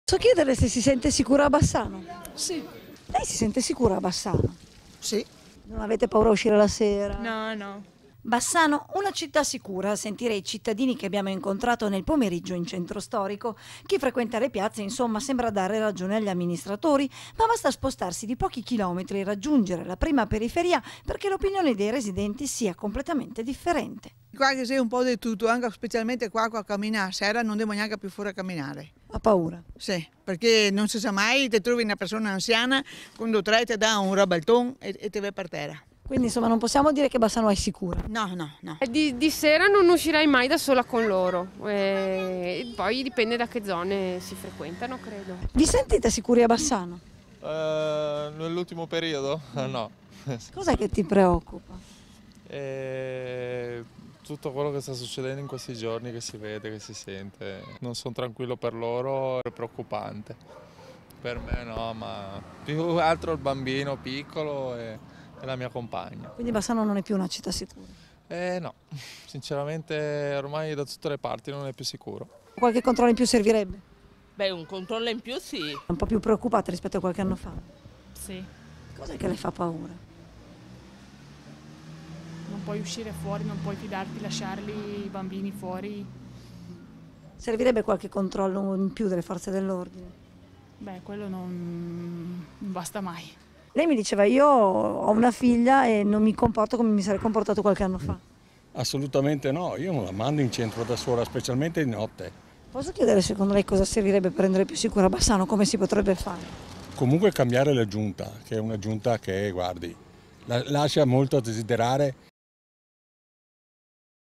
TG BASSANO (12/09/2017) - BASSANO E’ SICURA? PAROLA AI CITTADINI
TG BASSANO (martedì 12 settembre 2017) - Bassano? una città sicura a sentire i cittadini che abbiamo incontrato nel pomeriggio in centro storico. Chi frequenta le piazze, insomma, sembra dare ragione agli amministratori, ma basta spostarsi di pochi chilometri e raggiungere la prima periferia perchè l’opinione dei residenti sia completamente differente